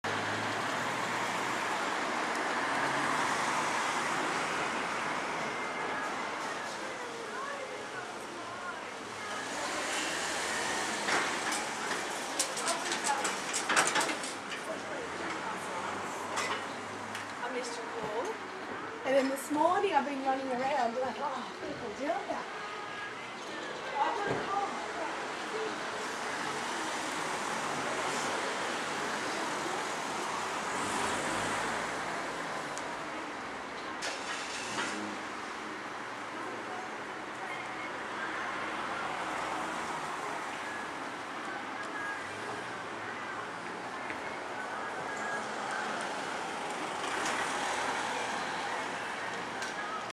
Wombat cross in Elwood (south of Melbourne)
Automobile have to slow down there